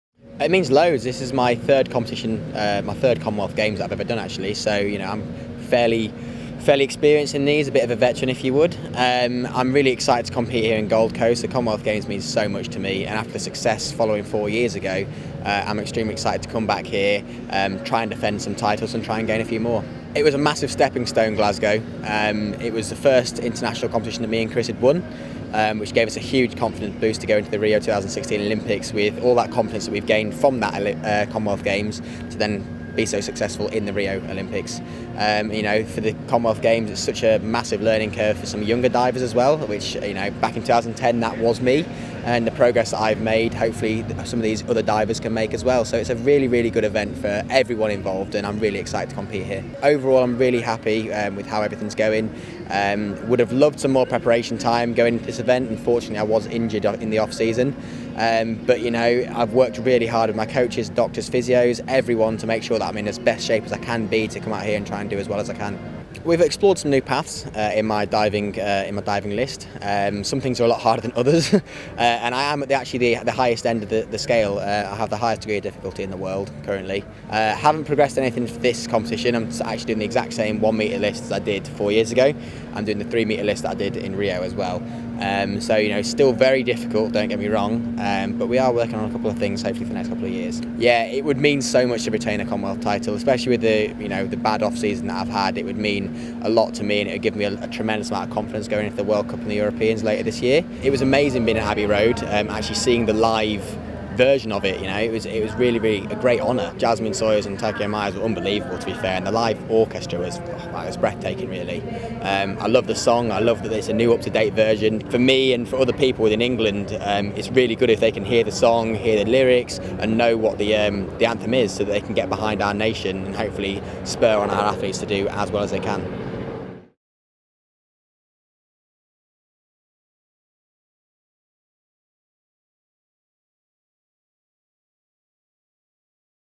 Jack Laugher eyes important defence of Commonwealth Games titles
Team England diver Jack Laugher believes it would be a great achievement to defend his two Commonwealth Games titles from Glasgow 2014, as well as adding another in the process.